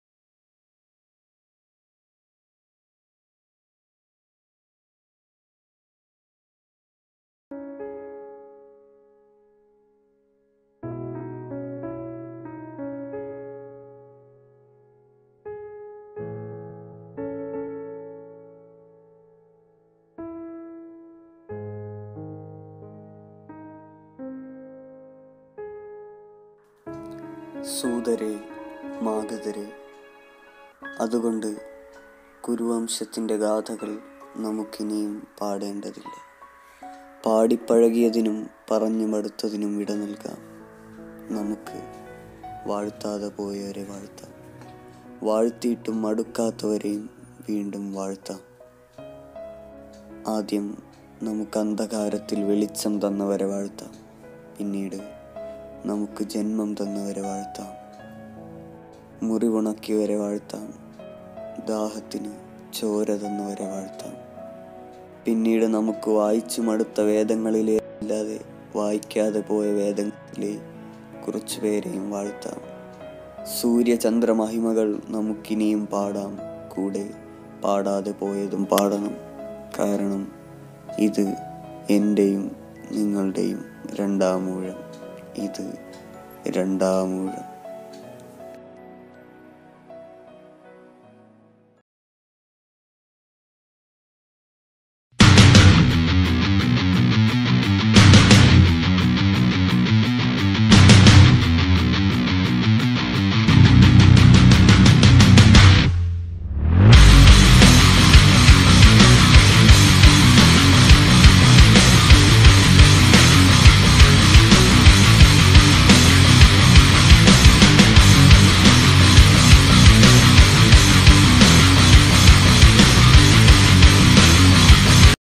Randamoozham | College magazine | Government college Kariavattom | Magazine title launch
Title launching video of The College magazine 'Randamoozham' of Government College kariavattom , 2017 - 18